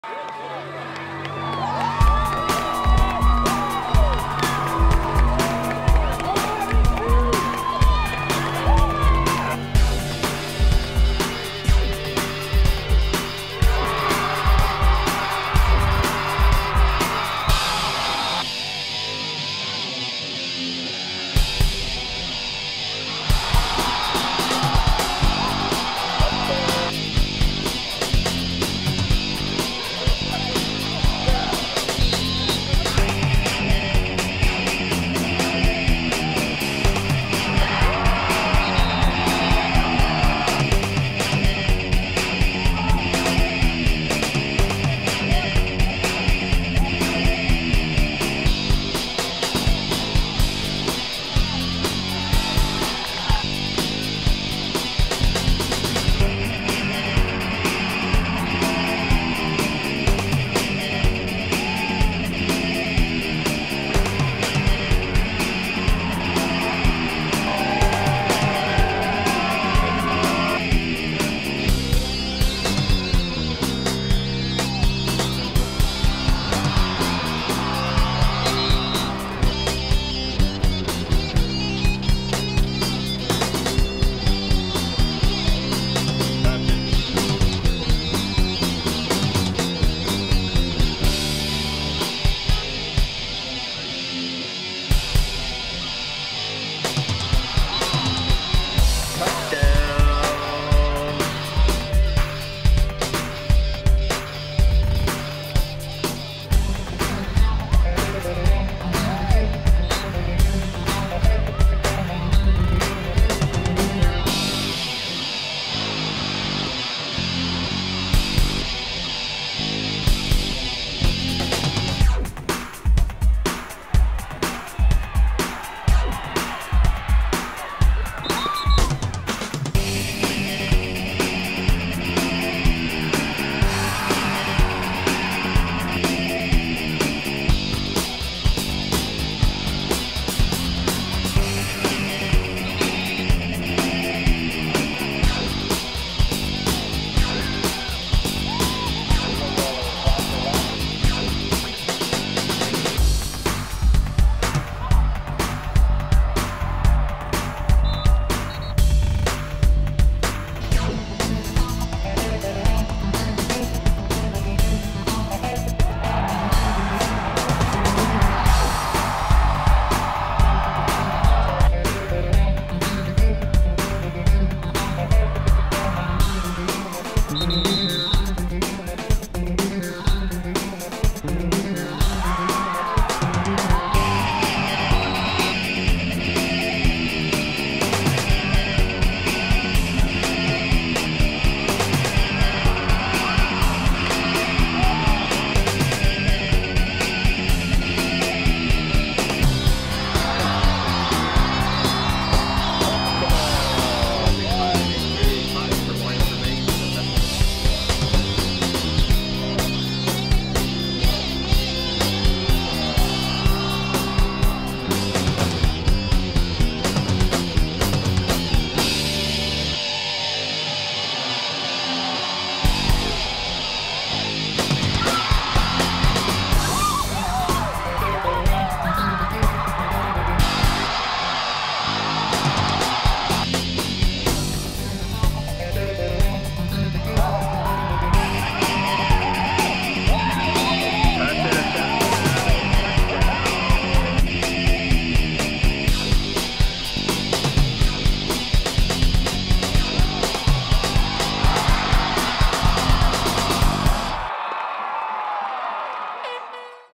2009 SUU vs UND Football highlights
The SUU Thunderbirds celebrate homecoming with a 35-10 rout of the North Dakota Fighting Souix on October 24, 2009 in the Coliseum in Cedar City, Utah.